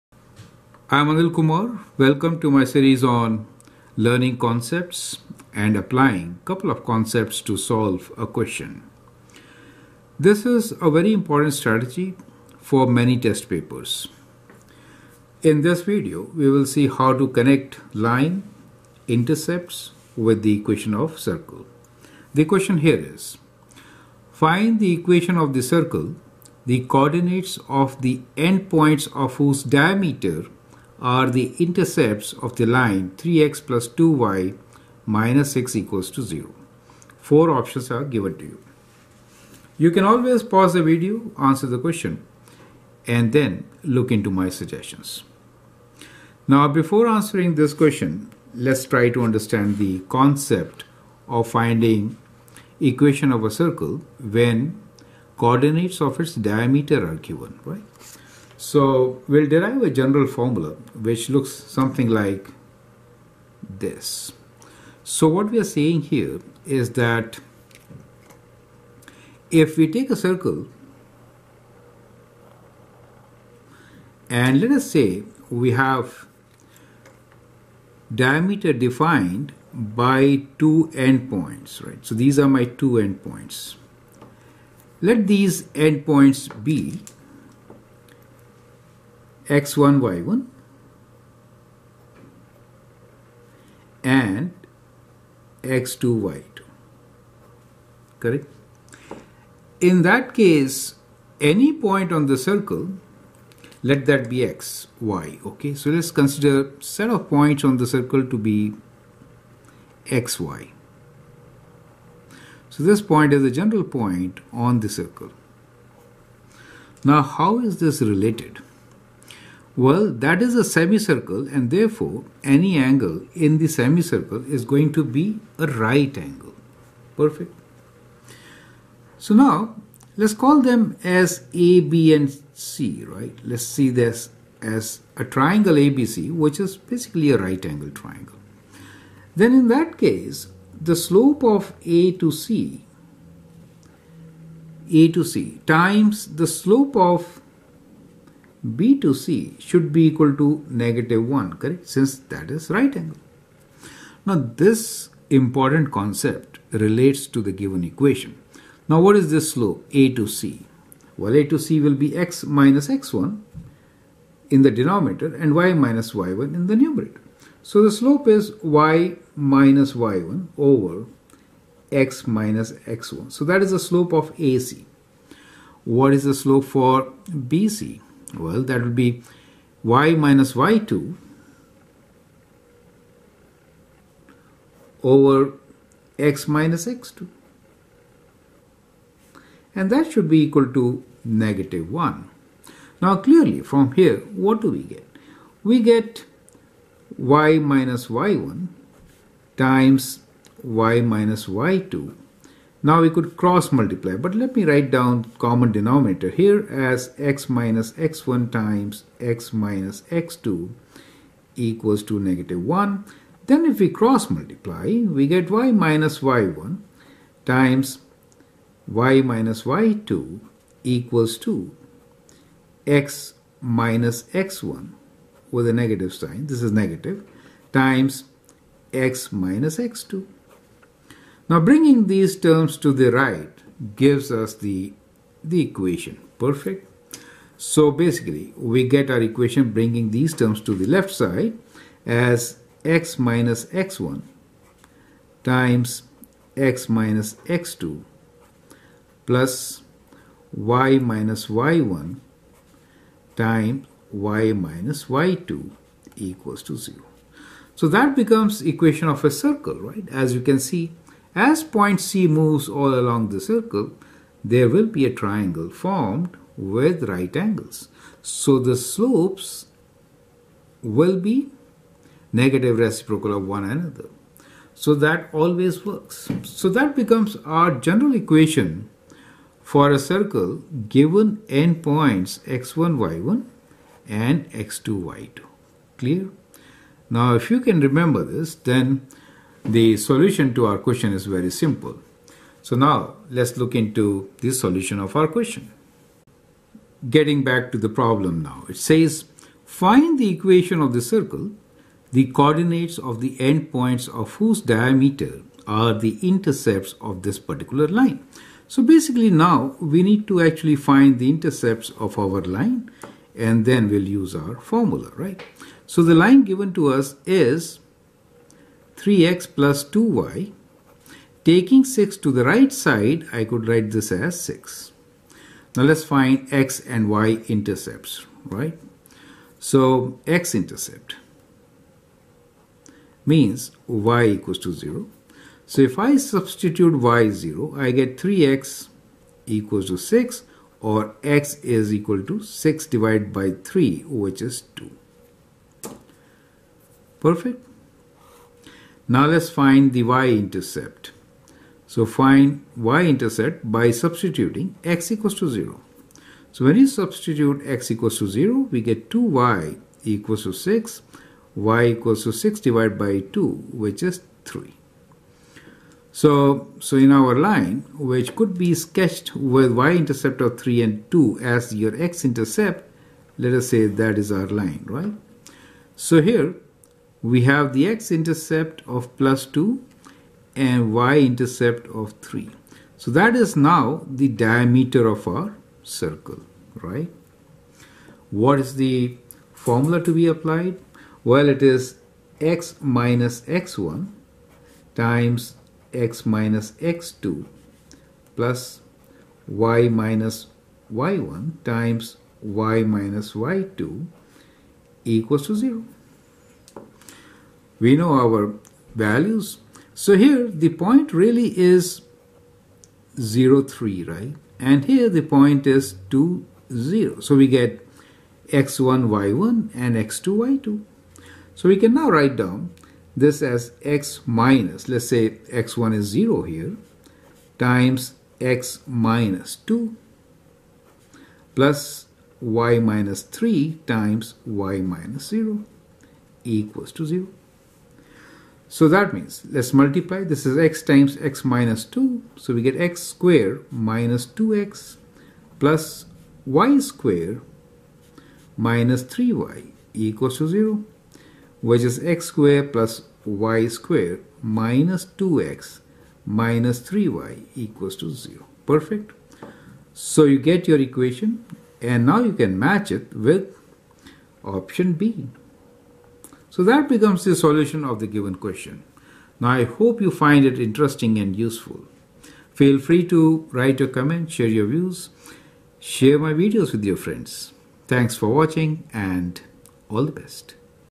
Find Equation of the Circle from Intercepts of a Line Segment as Diameter -GCSE-EDEXCEL-SAT
Find the equation of the circle, the coordinates of the end-points of whose diameter are the intercepts of the line 3x + 2y – 6 = 0.
a. x2 + y2 – 3x – 2y = 0
b. x2 + y2 – 2x – 3y = 0
c. x2 + y2 – 6x – 3y = 0
d. x2 + y2 – 3x – 6y = 0